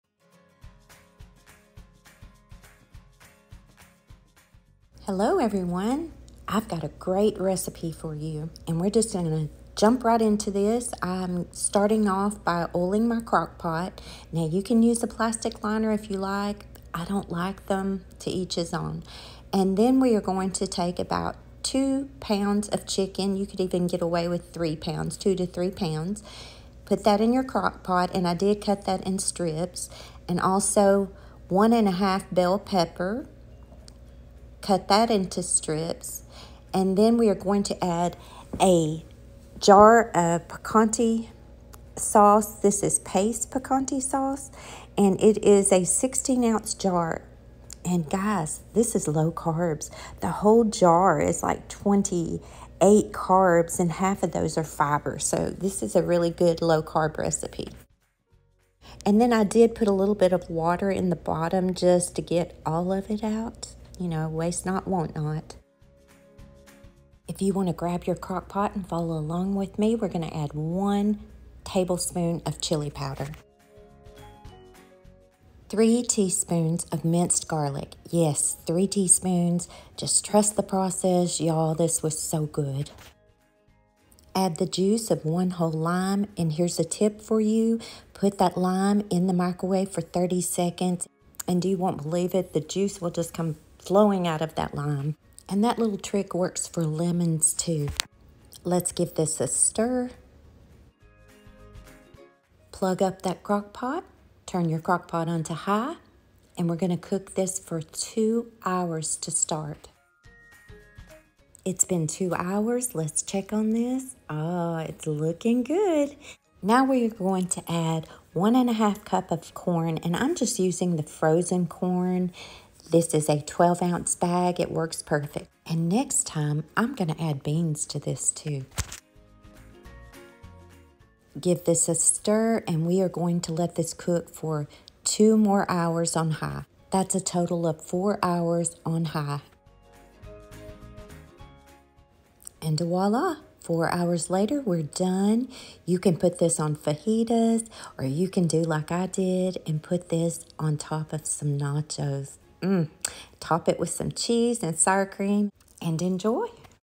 ‘Easy, Scrumptious Crockpot Recipe / Another great crockpot meal for you!
Good Morning FB and YouTube friends! I have another great low carb crockpot meal for you and your family to try,
… well if you omit the corn that is.  You could always add a mother low carb veggie. ( I had to carb it up with those nachos too. 🤣) This was so good y'all.
     I found this recipe in an old cookbook, but like some of you I put my own twist on this one.  This recipe was a keeper!
     Don't you just love easy crockpot meals?  I certainly do, especially in the summertime when you would rather be outside enjoying the sunshine or whatever it is that you would rather be doing.
       Well time for me to get busy.  Y'all have a beautiful day! ❤️Deb

Ingredients:
2-3 lbs chicken
1 1/2 bell peppers any color
1 whole lime or about 3 tsp of lime juice
1 tbsp chili powder
3 tsp minced garlic
1 16 oz jar of Pace Picante Sauce
1 1/2 C frozen whole kernel corn  or a low carb veggie of your choice
Toppings: Shredded mild cheddar and sour cream  ( and onions or beans would be a great addition as well)
This can be eaten on nachos, as fajita's or even as a stew! Yum😋